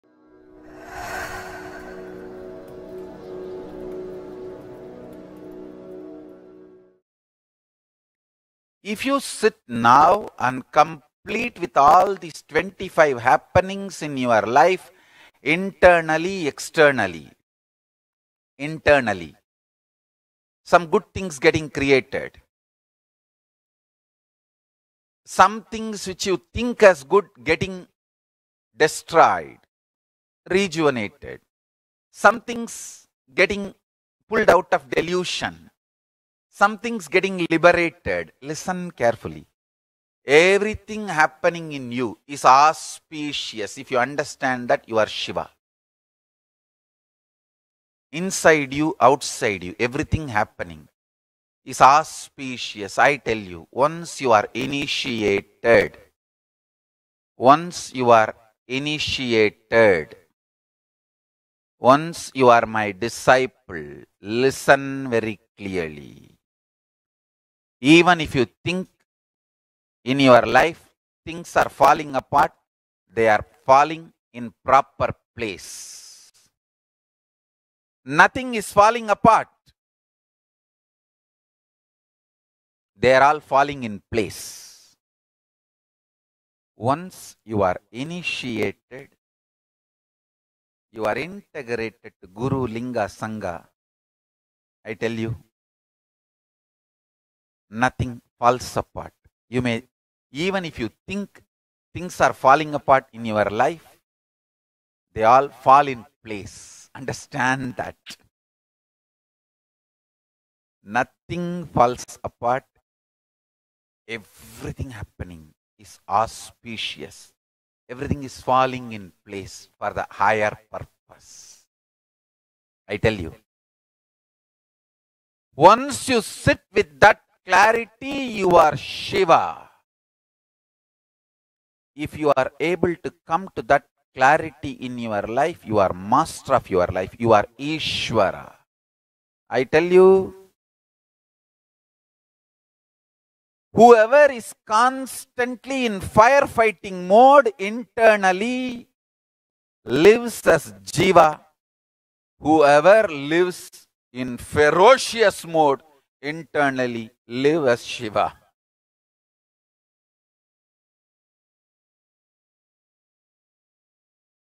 Once Inititated nothing Falls Apart Everything Falls in Place
If you sit now and complete with all these 25 happenings in your life internally-externally. Internally, some good things getting created, some things which you think as good getting destroyed, rejuvenated, some things getting pulled out of delusion, some things getting liberated. Listen carefully everything happening in you is auspicious, if you understand that, You are Shiva, inside you outside you everything happening is auspicious, I tell you once you are initiated, once you are initiated, once you are my disciple, listen very clearly even if you think in your life things are falling apart, they are falling in proper place, nothing is falling apart, they’re all falling in place. Once you are initiated you are integrated to Guru-Linga-Sangha, I tell you nothing falls apart, you may, even if you think things are falling apart in your life, they all fall in place understand that! Nothing falls apart everything happening is auspicious, everything is falling in place for the higher purpose. I tell you, once you sit with that clarity You are Shiva, if you are able to come to that clarity in your life, you are master of your life, you are Ishwara. I tell you whoever is constantly in fire fighting mode internally lives as jiva, whoever lives in ferocious mode internally lives as Shiva.